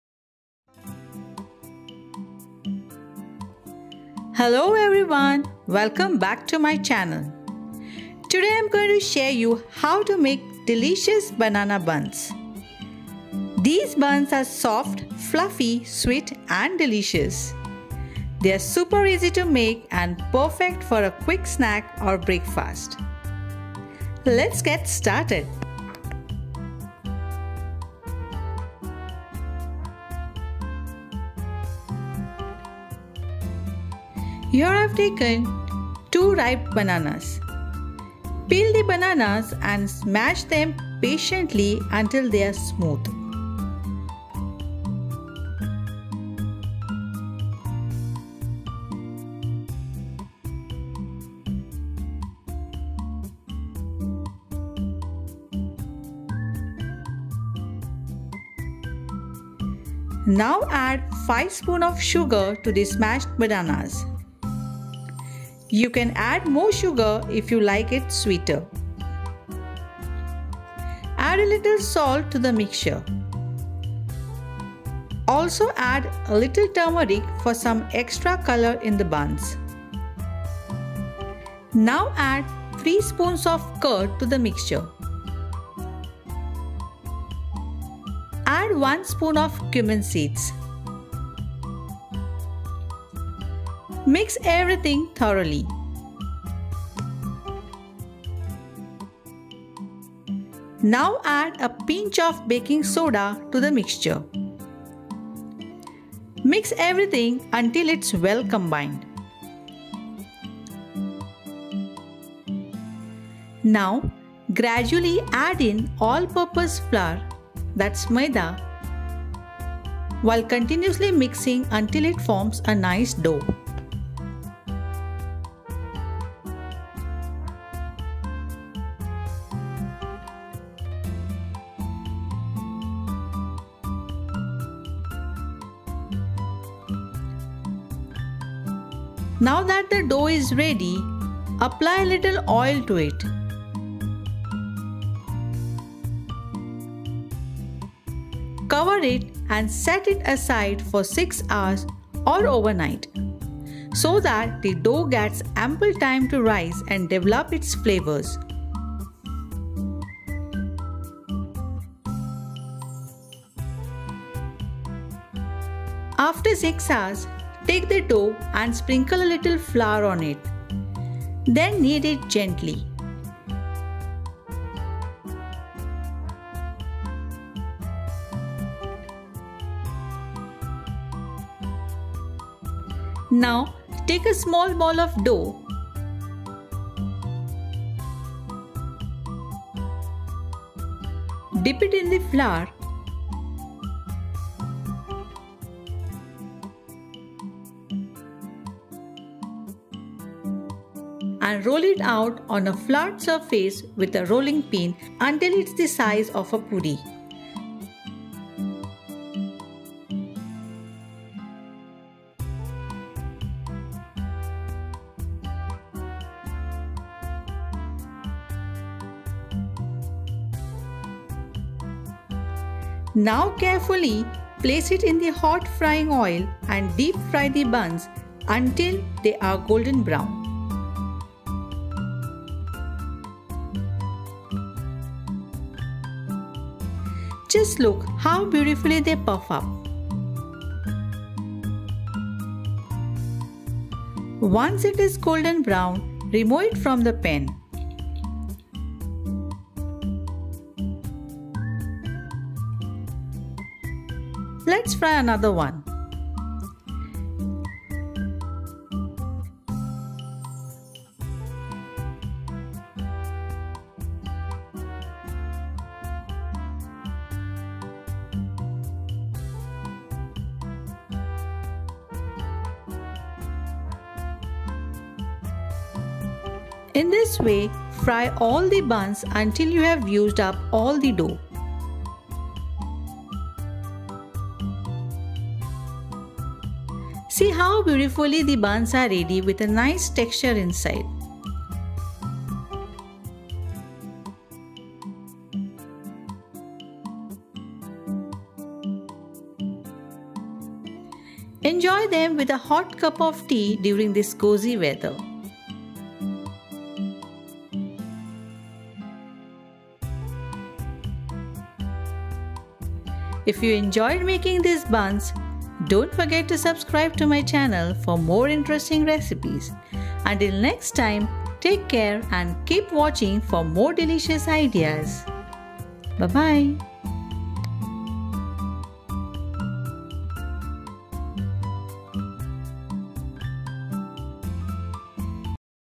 ACT FAST: Secret of Banana Buns - Make Yours Today
Learn how to bake perfect, fluffy buns with my easy, step-by-step recipe! This will guide you through the process to achieve delicious, homemade buns every time.

Watch Now!!